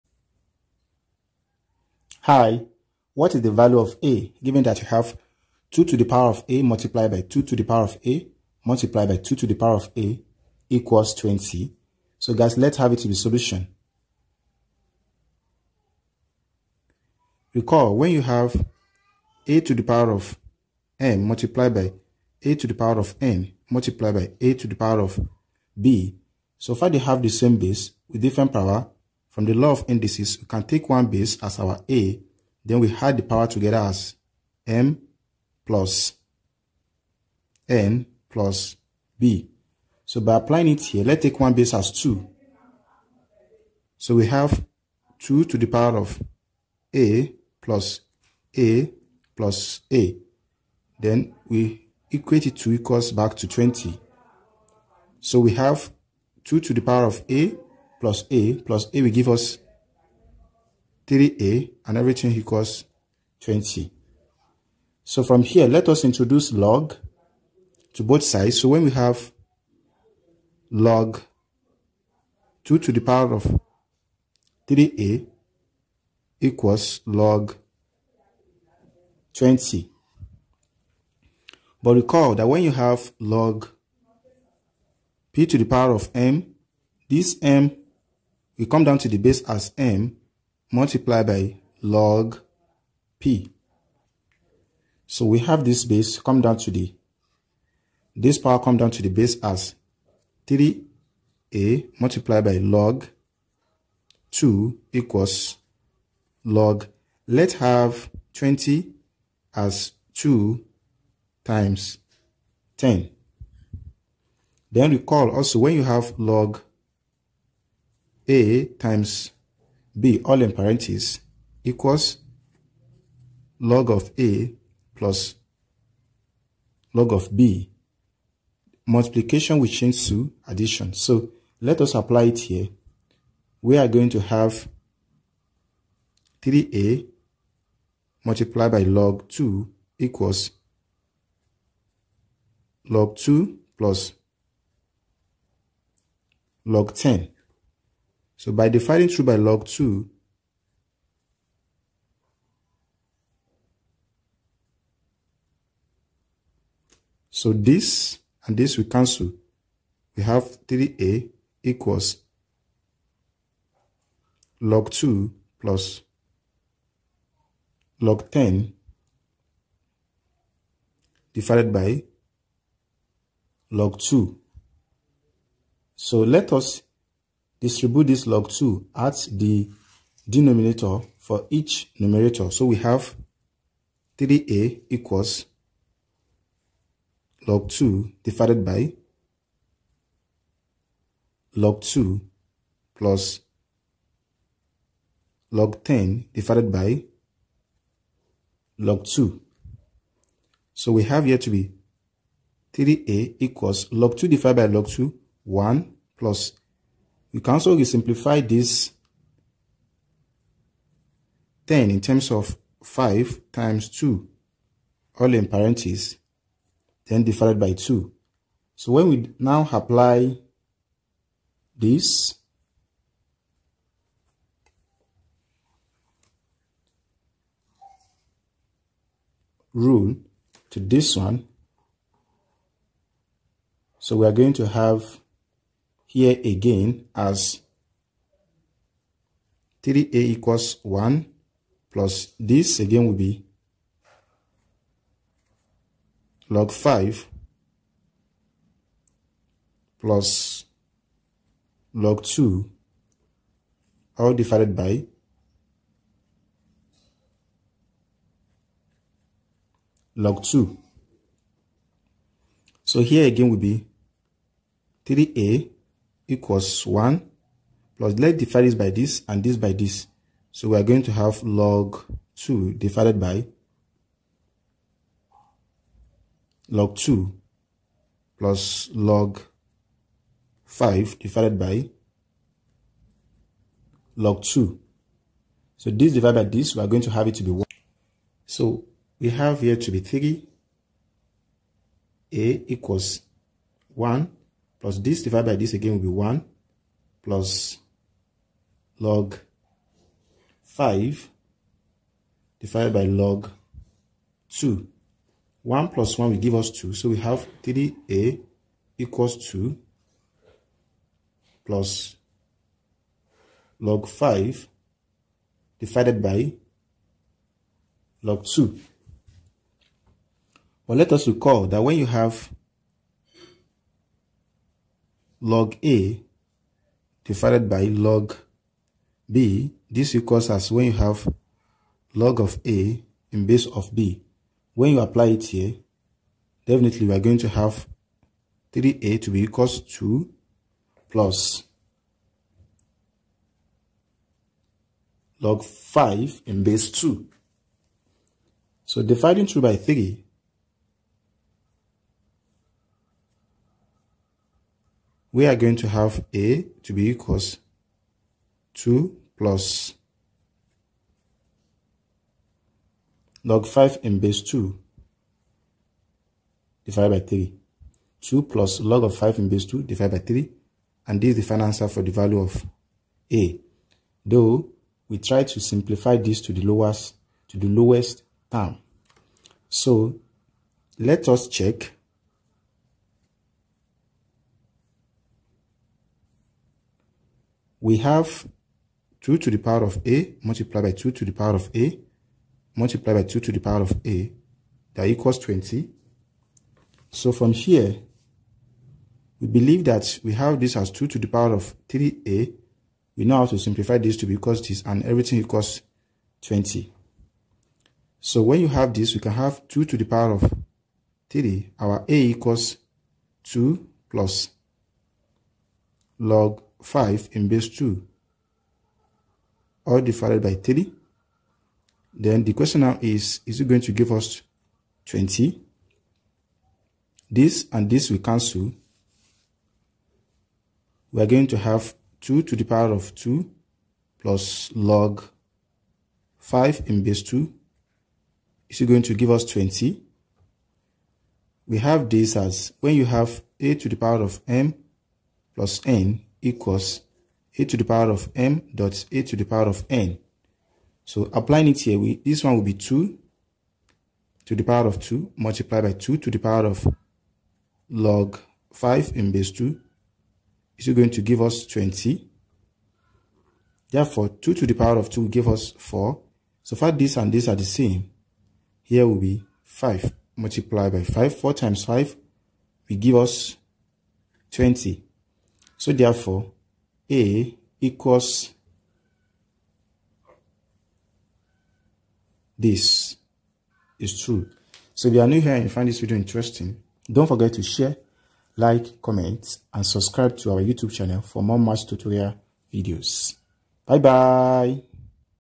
USA Olympiad Exponential Equation: solve for a=?
Explore the interesting world of exponential equations with varying bases and unlock the power of exponentiation! In this educational video, we delve into the concept of exponential equations where the base is not a constant value. Learn the fundamentals, discover the properties, and master solving equations with different bases, all explained clearly and concisely. Join us on this exponential journey and level up your mathematical prowess!

solvingexponentialequationsusing logarithms
solvingexponentialequationswithdifferent bases